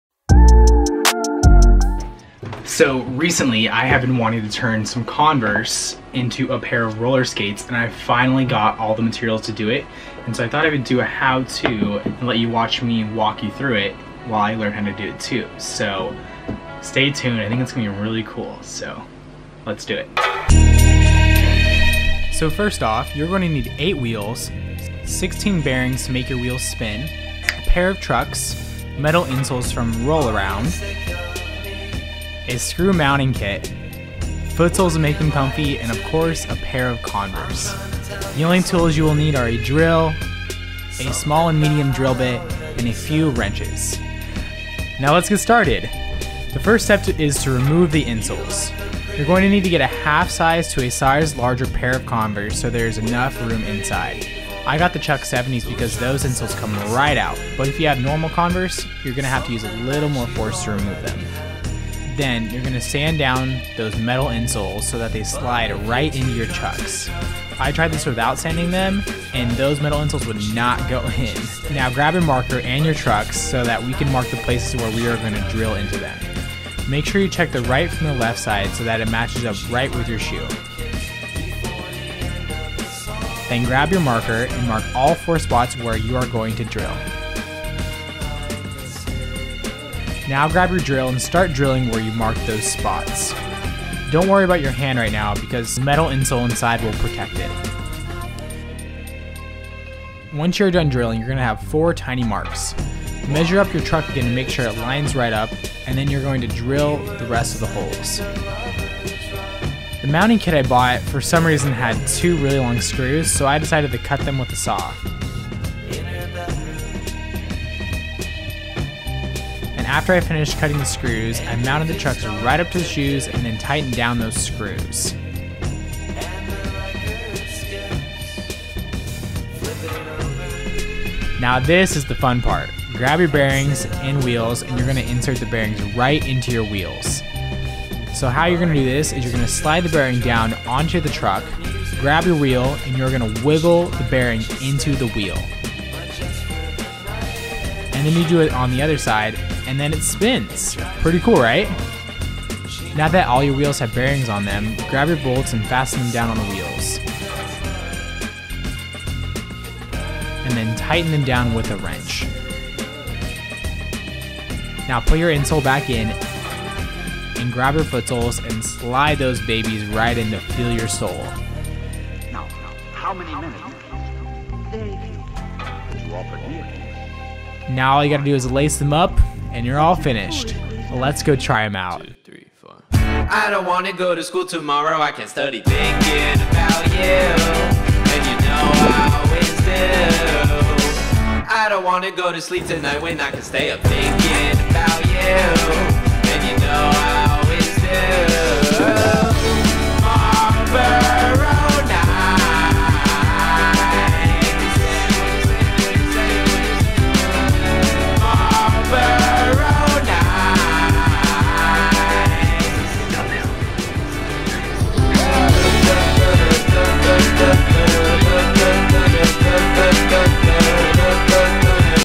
How To Make Converse Roller Skates!!
Hey Friends!! For so long I have been wanting to turn a pair of Converse into a pair of roller skates and I finally did it!!! They turned out so cool and actually work. Now all I need to practice is my roller skating ability hahaha!

If you follow this how to make sure to tag me in it so that I can see your masterpieces!! @jacobzander_

Option #1 Supplies (Cost Effective):
If you are lucky enough to find a pair of roller skates your size at a thrift store you can remove the bottom off of them (trucks & wheels) and use those trucks and wheels instead of buying them new. I went to like 5 thrift stores, but couldn't find my size anywhere so I went with option #2 below haha! If you go this way all you will need are the metal insoles and possibly the mounting kit :)

Metal Insoles (Choose a half size down because Converse run big)
Converse (Choose a half to size larger than what you would normally wear)

✧ FAQ ✧
+ what do you use to edit videos? Adobe Premiere Pro